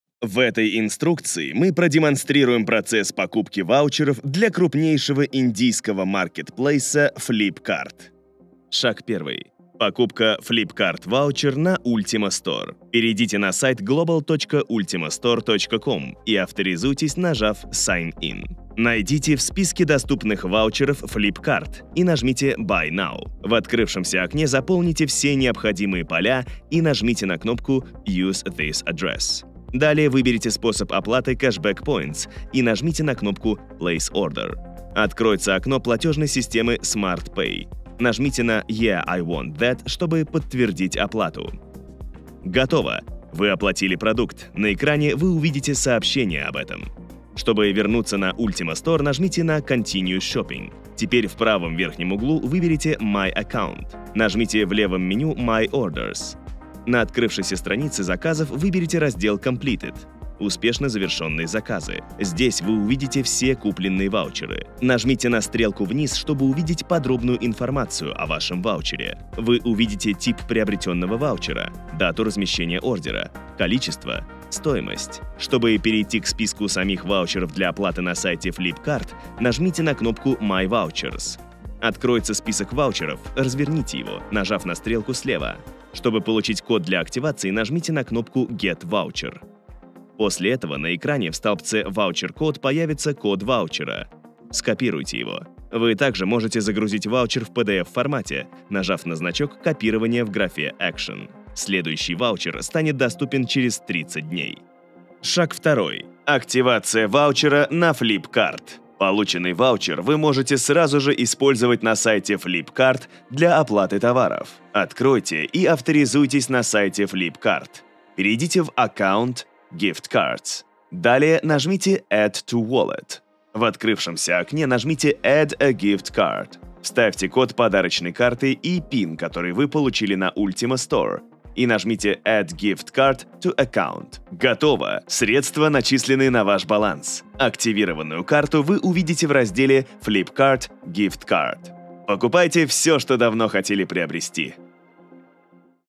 Как купить и активировать ваучер для Flipkart
Flipkart — ведущий онлайн-маркетплейс в Индии. Он предлагает широкий ассортимент товаров, включая электронику, одежду, косметику, бытовую технику.

В этой инструкции мы продемонстрируем вам весь процесс покупки ваучеров для Flipkart. Вы узнаете:
✔ как купить Flipkart-ваучер на Ultima Store;
✔ где найти код активации ваучера;
✔ как активировать ваучер на Flipkart.